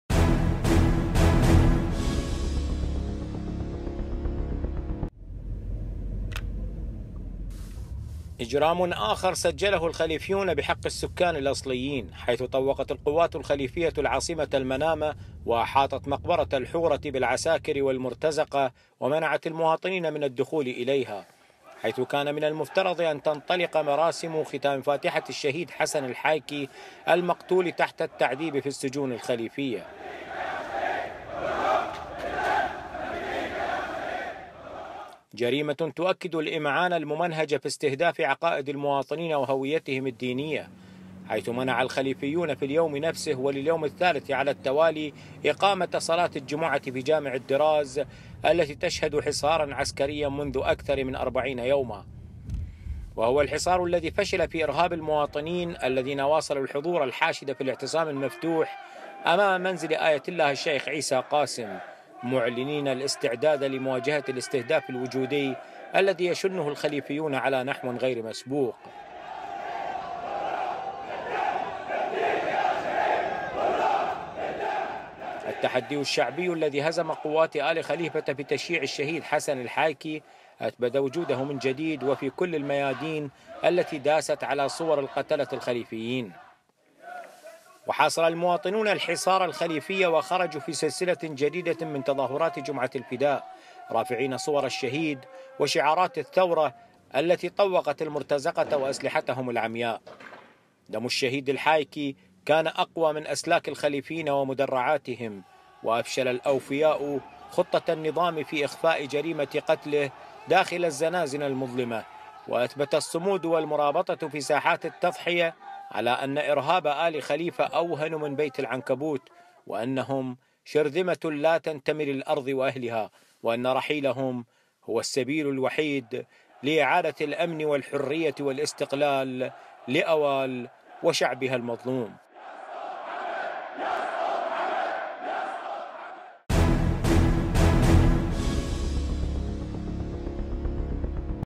تقرير متلفز: منع مراسم ختام فاتحة الشهيد الحايكي: حرب الوجود والهوية
إجرامٌ آخر سجّله الخليفيون بحقِّ السكان الأصليين، حيث طوّقت القوات الخليفية العاصمة المنامة، وأحاطت مقبرة الحورة بالعساكر والمرتزقة ومنعت المواطنين من الدّخول إليها، حيث كان من المفترض أن تنطلق مراسم ختام فاتحة الشّهيد حسن الحايكي المقتول تحت التعذيب في السجون الخليفية.

جريمة تؤكد الإمعان الممنهج في استهداف عقائد المواطنين وهويتهم الدينية، حيث منع الخليفيون في اليوم نفسه، ولليوم الثالث على التوالي؛ إقامة صلاة الجمعة في جامع الدراز التي تشهد حصاراً عسكريا منذ أكثر من أربعين يوماً، وهو الحصار الذي فشل في إرهاب المواطنين الذين واصلوا الحضور الحاشد في الاعتصام المفتوح أمام منزل آية الله الشيخ عيسى قاسم، معلنين الاستعداد لمواجهة الاستهداف الوجودي الذي يشنه الخليفيون على نحو غير مسبوق، وباستعمال كلِّ أدوات القتل والاقتلاع والإبادة الجماعيّة.

التحدي الشعبي الذي هزم قوات آل خليفة في تشييع الشهيد حسن الحايكي، أثبت وجوده من جديد وفي كلّ الميادين التي داست على صور القتلة الخليفيين، وحاصرَ المواطنون الحصارَ الخليفيّ وخرجوا في سلسلةٍ جديدة من تظاهرات "جمعة الفداء"، رافعين صور الشهيدِ وشعارات الثّورة التي طوّقت المرتزقة وأسلحتهم العمياء.

دم الشهيد الحايكي كان أقوى من أسلاك الخليفيين ومدرعاتهم، وأفشل الأوفياءُ خطّة النظام في إخفاء جريمة قتله داخل الزنازن المظلمة، وأثبت الصمودُ والمرابطة في ساحات التّضحيةِ على أنّ إرهاب آل خليفة أوهنُ من بيت العنكبوت، وأنهم شرذمةٌ لا تنتمي للأرضِ وأهلها، وأنّ رحيلهم هو السّبيلُ الوحيد لإعادة الأمن والحرية والاستقلال لأوالَ وشعبها المظلوم.